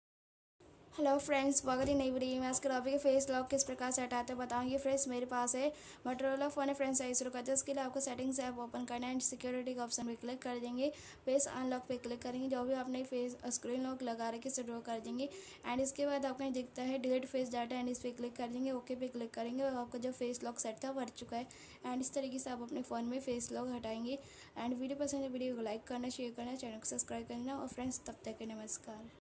Moto g52 face lock full setting | how to remove face lock | face lock Kaise hataye
Google search
Techno Puja Technopuja@Technopuja